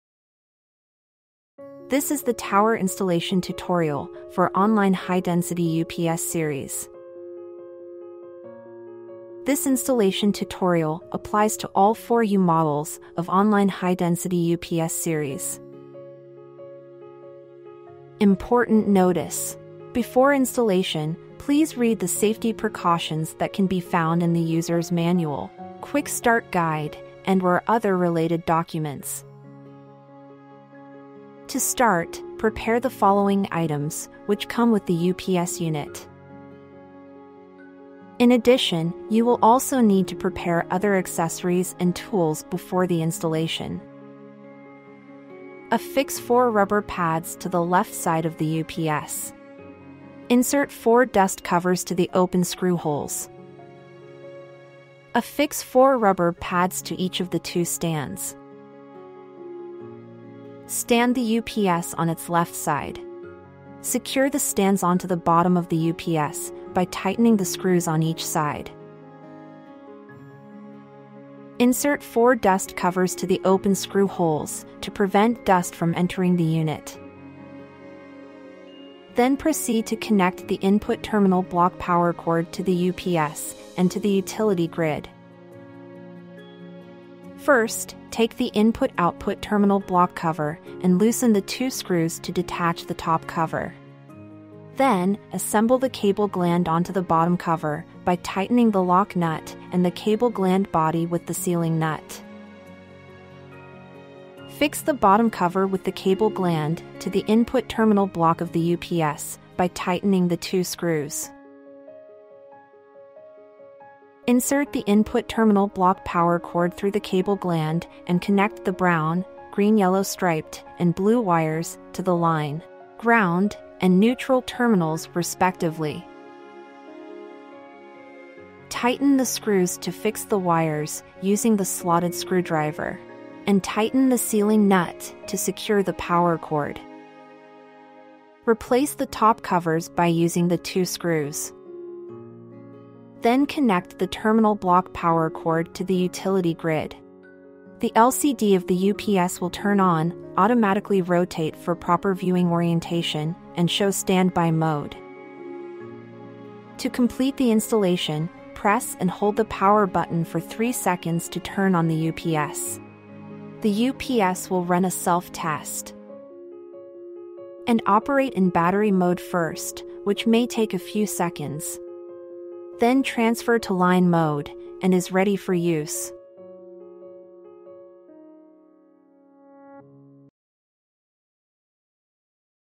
CyberPower Online (High-Density) UPS Series (4U) for Tower Product Installation Tutorial
Step-by-step tower installation tutorial for Online (High-Density) UPS Series (4U).

Before installation, please read the safety precautions that can be found in the user's manual, quick start guide and/or other related documents.

0:00 Introduction
0:18 Important Notice
0:31 Preparation
0:45 Tower Installation
1:19 Connecting Input Terminal Block Power Cord